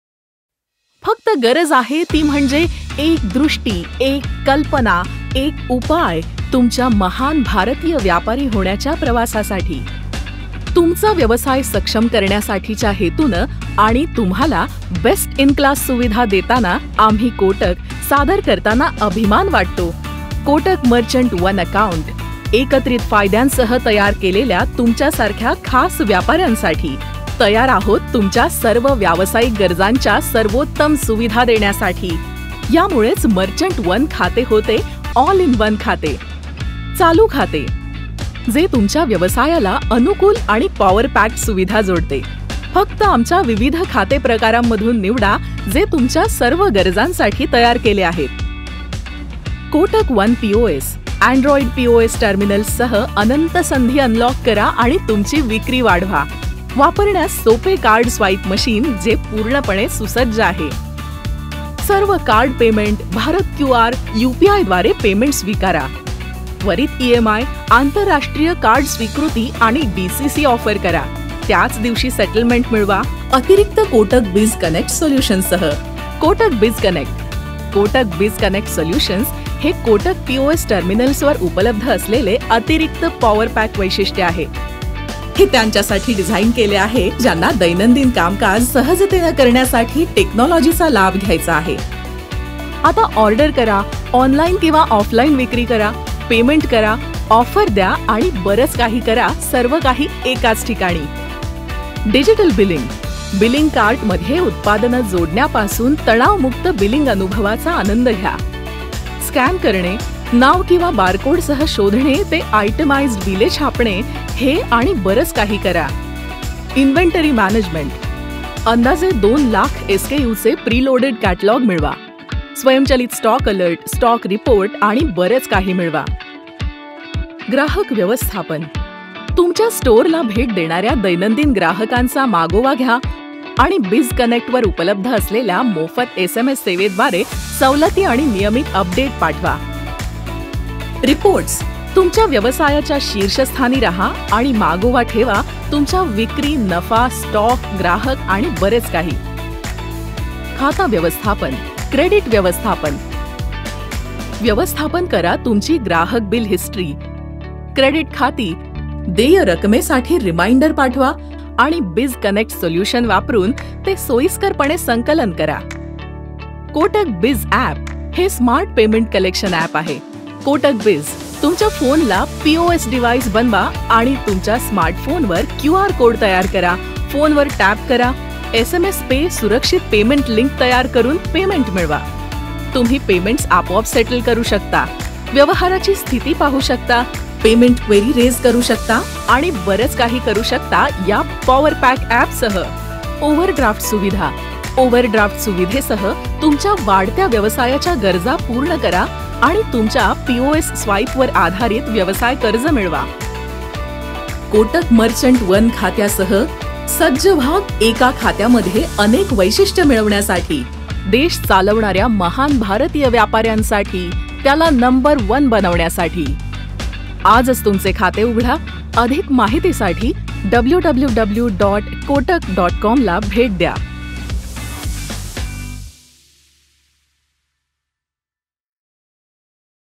ऑल-ईन-वन खाते, महान भारतीय व्यापाऱ्यांसाठी
तुमच्या बिझनेसच्या सर्व आवश्यकतांसाठी एक सक्षम खातं हवं आहे का? मर्चन्ट वन चालू खातं व्यापारी/
रिटेलर्सना एकाच जागी अनेक सेवा देतं. जसं: चालू खातं, पेमेंट सोल्युशन्स, कस्टमर डेटाबेस मॅनेजमेंट,
इन्व्हेंटरी आणि कॅश मॅनेजमेंट, ओवरड्राफ्ट फॅसिलिटी आणि बरंच काही. चला, मर्चन्ट वन खात्यामधून वनची
सक्षमता मिळवा.